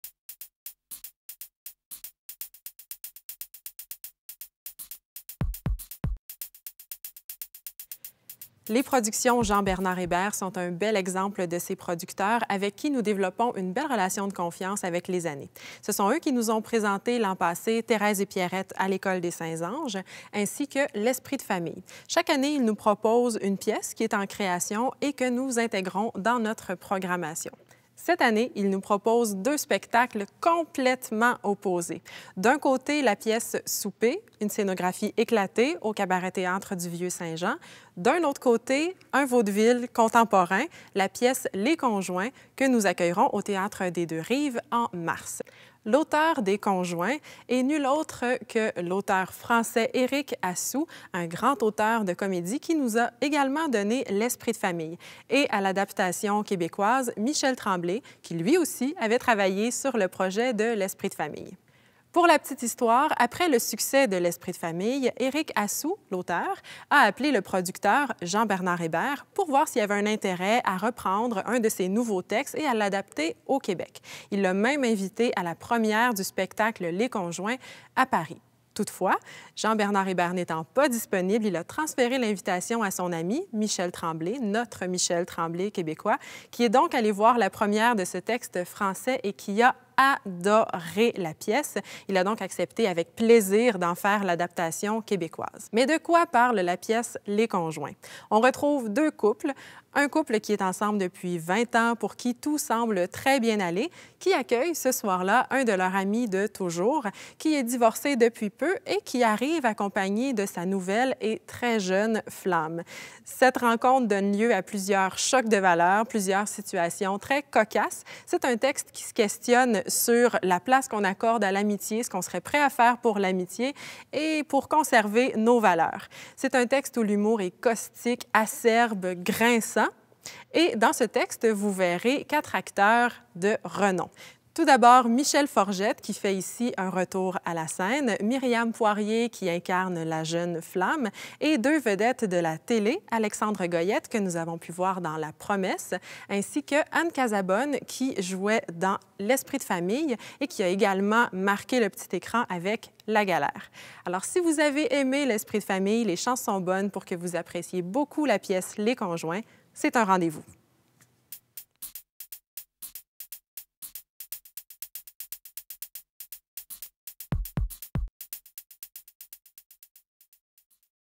Les conjoints
La pièce Les Conjoints sera présentée au Théâtre des Deux Rives le 29 mars 2013 à 20h.
Informations et billetterie ou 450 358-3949 sans frais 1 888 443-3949